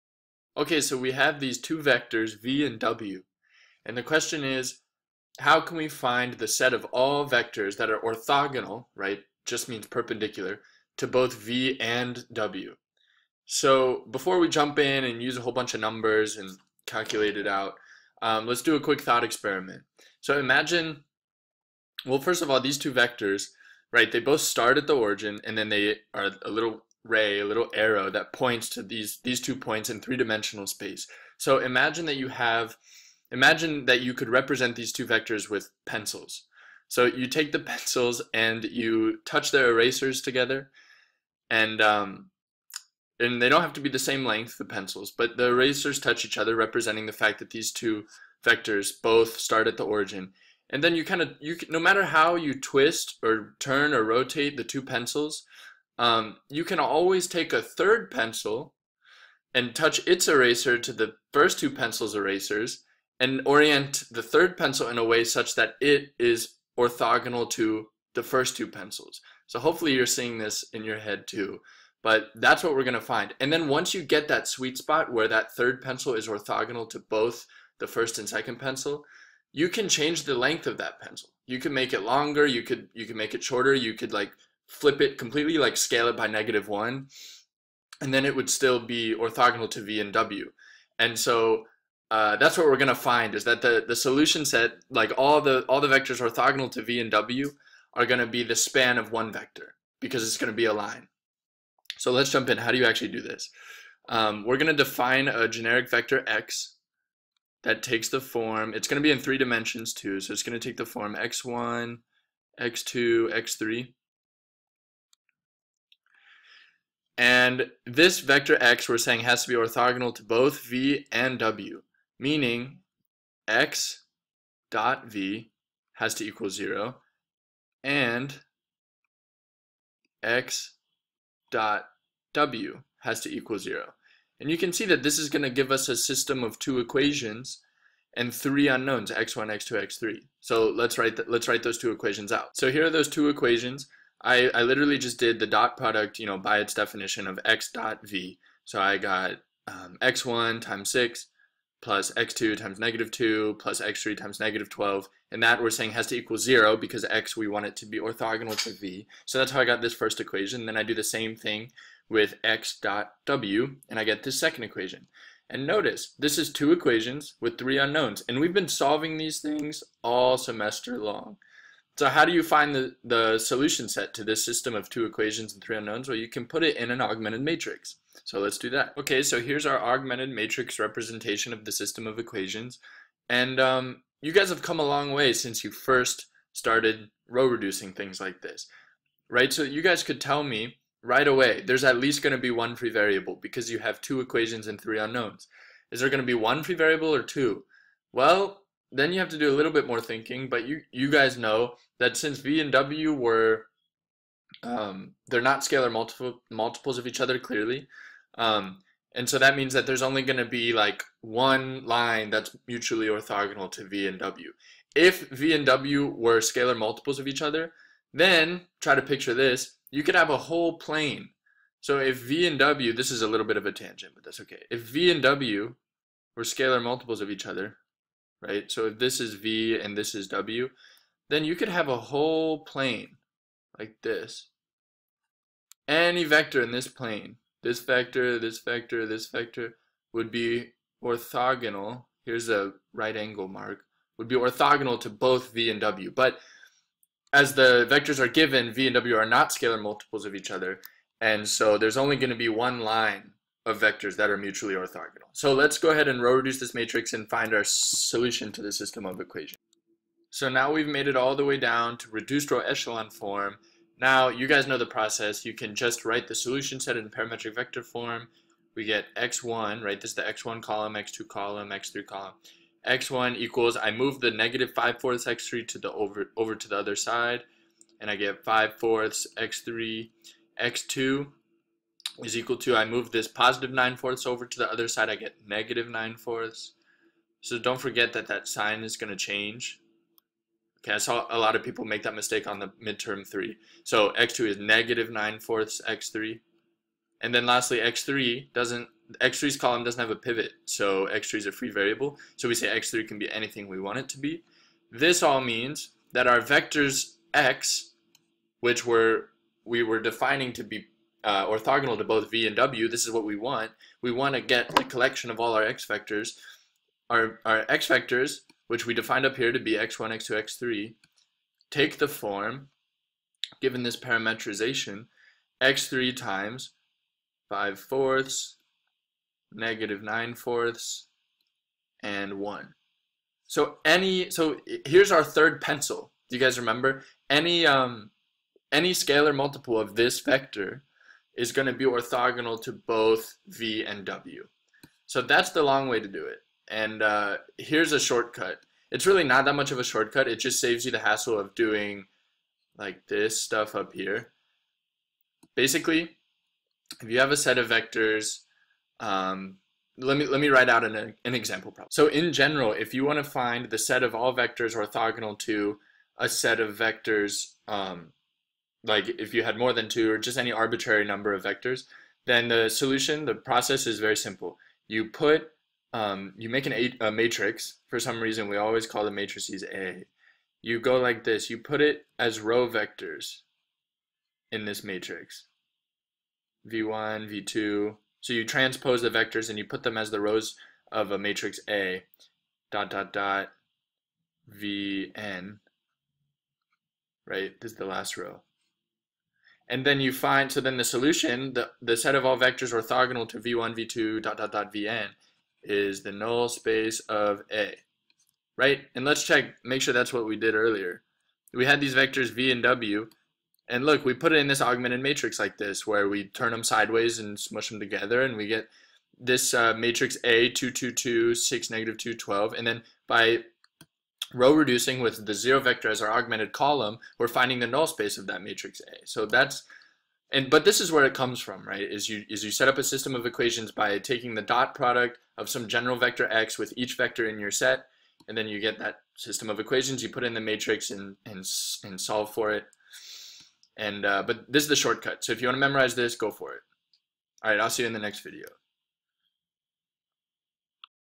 How to Find All Vectors Orthogonal to v and w [Passing Linear Algebra]
Skip the pencil thought experiment: 1:41